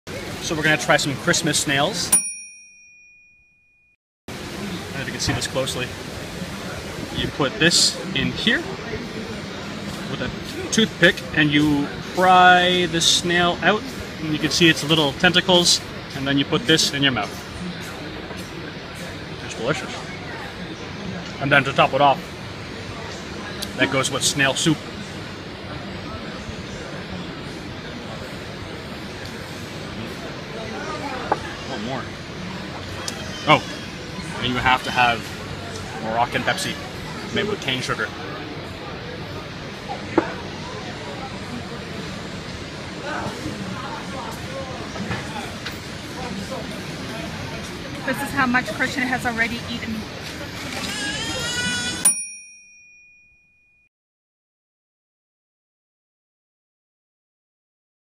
WEWANNAGO MOROCCO: Eating snails in Fes
We could’ve found an American restaurant and had a western meal, but when traveling… Why not eat like the locals? Let’s call this my review of traditional Moroccan snails and soup. Whether you’re in Fes, Essaouira or Marrakech, hot snail soup is never far. We found a street food vendor just outside the main gate to the bazaar. At about $0.50 (USD) you get 30-50 snails, it’s a bargain and it will fill you up. The snails themselves taste “earthy”. I think the taste is somewhere between cooked chicken and a prepared mushrooms. The shelled snails are actually tasty but the texture didn’t work well for me. I’m picky like that. The soup and broth however, were delicious! Again, a little bit “earthy” but full of flavour and salty! which is good. As a side note, any popular beverages like Pepsi are the Cane sugar or raw sugar versions. I don’t know if that’s healthier by any means but the sweetness is off the charts! Travel to Fez, you won't be sorry.

Christiaan & Kseniya Welzel

(Filmed on an iPhone and Steadicam Smoothee)